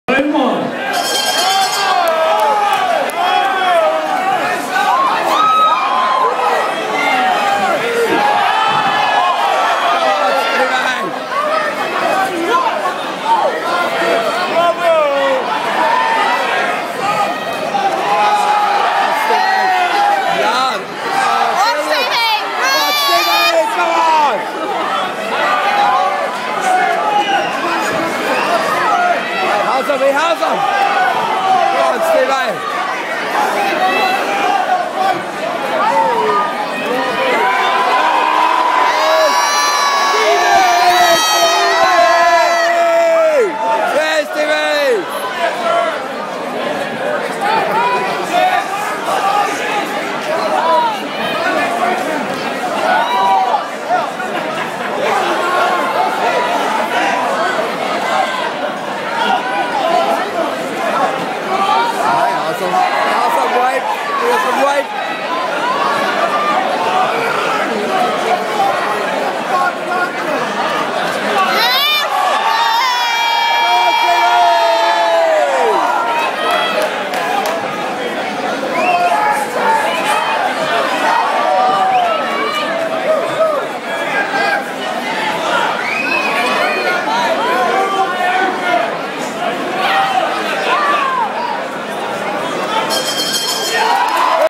Stephen McConnell NI Live events boxing.MOV
NI Live Events White Collar Boxing Kellys Nightclub Portrush Friday 25th March 2011